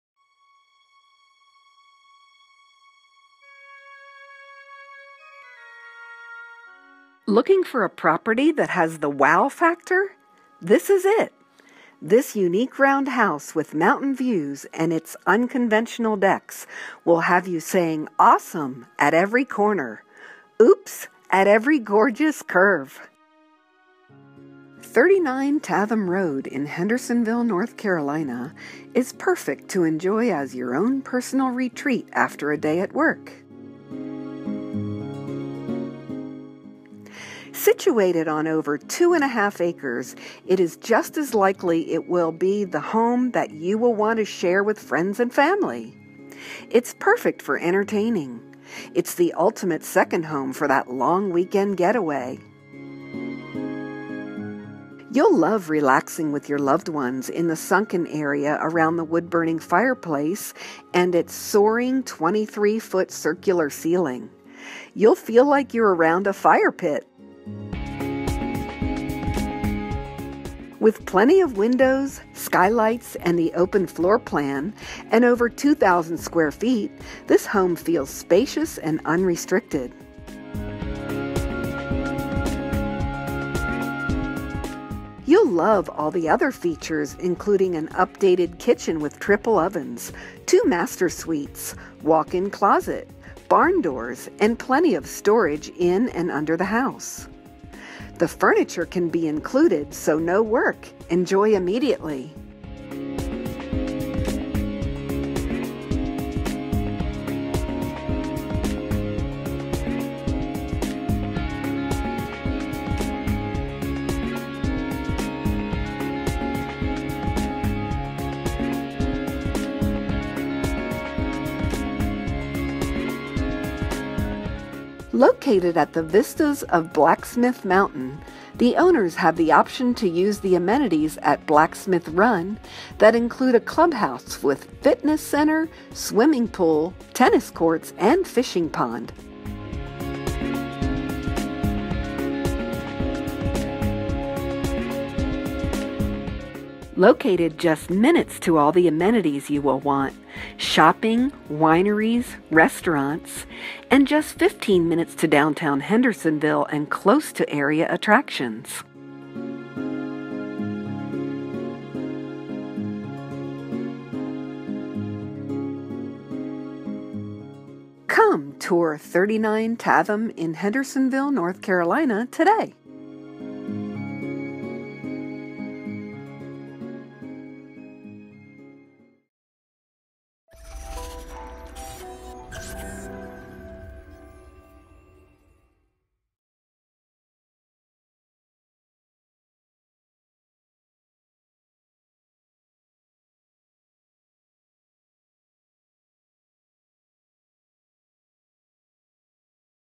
39 Tatham Road, Hendersonville, NC
Take a high definition video tour of this beautiful property for sale:
39 Tatham Road, Hendersonville, NC